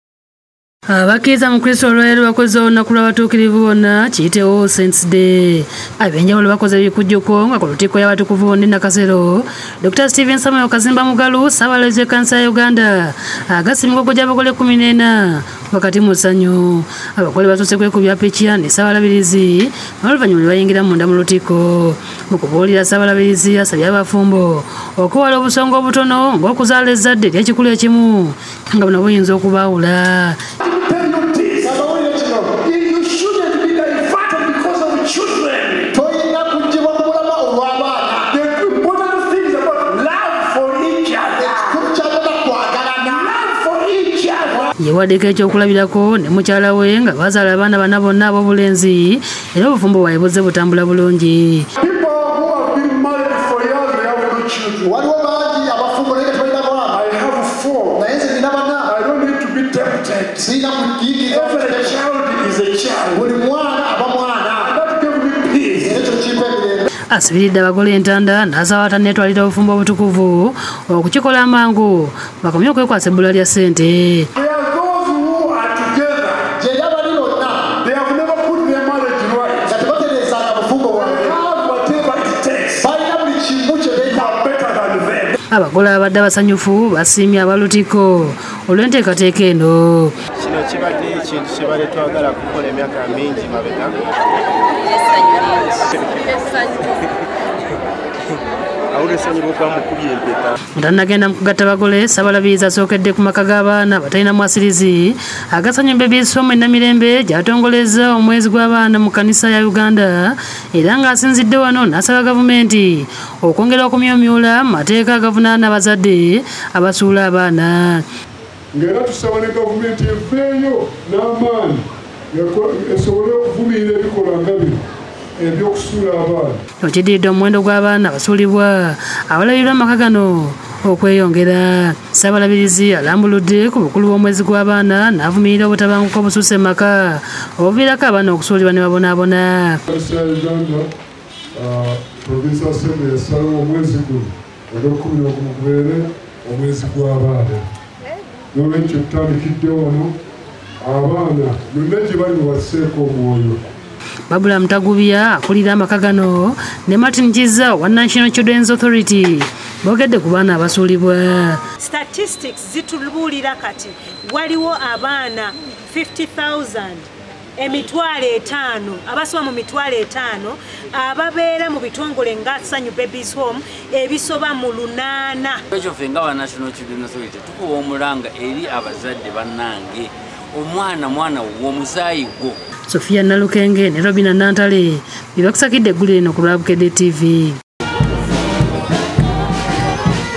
Agataliikonfuufu: SSABALABIRIZI KAZIMBA AGASSE EMIGOGO 14. ALAMBUDDE ABAANA
Dr. Samuel Steven Kazimba Mugalu ssaabalabirizi w’ekkanisa ya Uganda akuutidde abafumbo obuteesiba ku busonga obutono obuyinza okuviirako obufumbo bwabwe okufa.Abadde agatta emigogo gy’abagole  14 ku lutikko ya All saints e Nakasero.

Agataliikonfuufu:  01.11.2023
Sharifah Bakka Tamale.